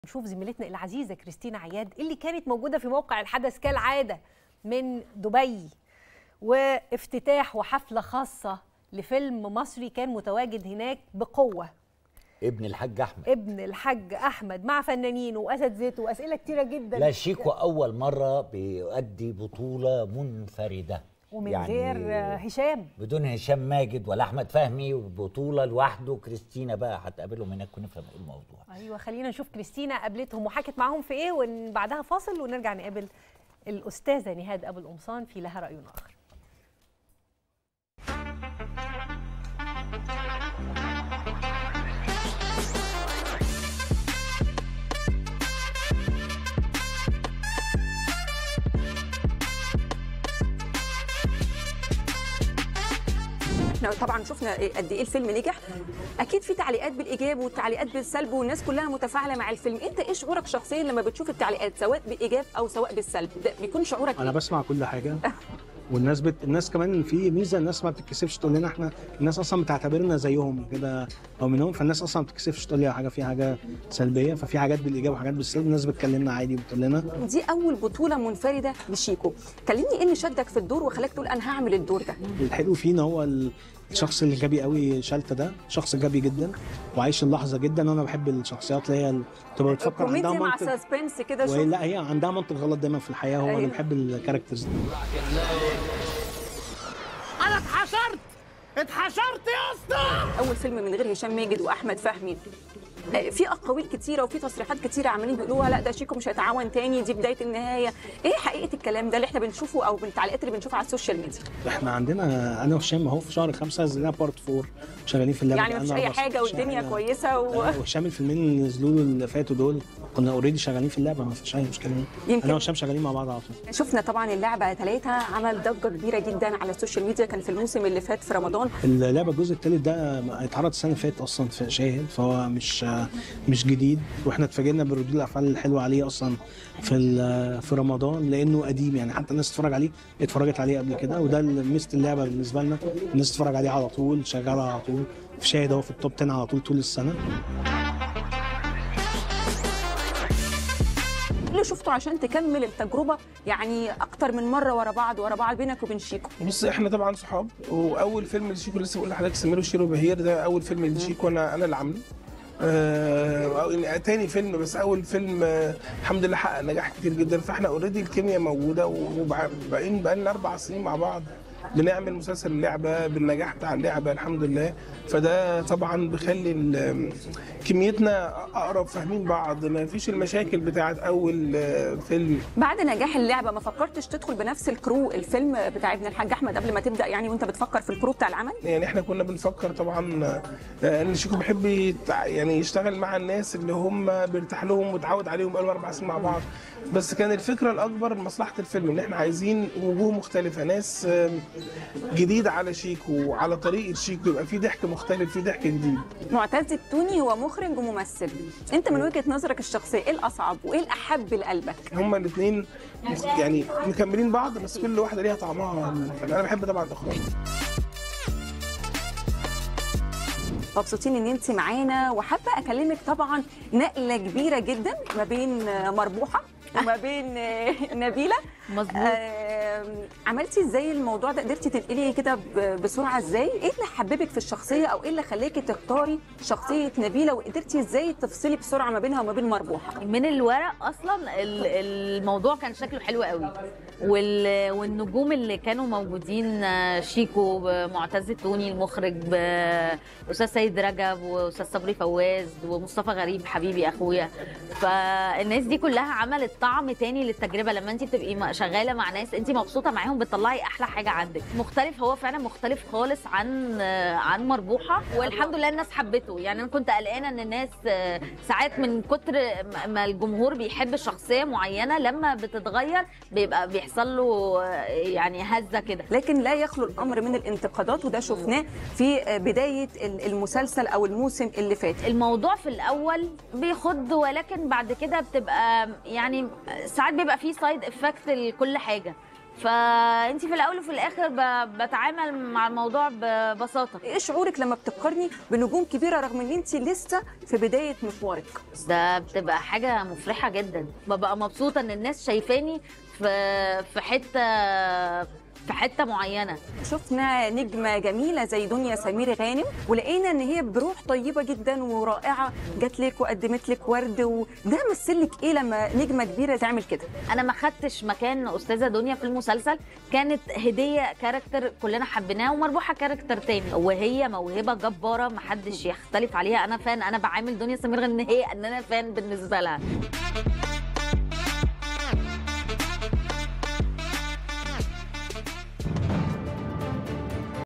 كاميرا حديث القاهرة والاعلامية كريستينا عياد في العرض الأول لفيلم "ابن الحاج أحمد" في دبي
كاميرا حديث_القاهرة والاعلامية كريستينا_عياد في العرض الأول لفيلم "ابن الحاج أحمد" في دبي .. ومقابلات حصرية مع النجوم شيكو و رحمة_احمد والمخرج